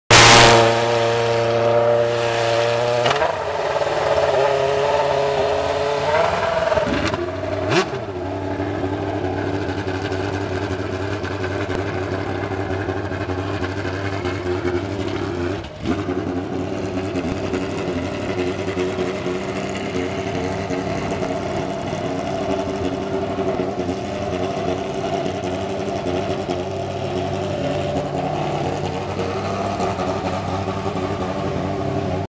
Lotus Renault R30 and Lotus Cosworth T125 fire up at the 2012 Goodwood Festival of Speed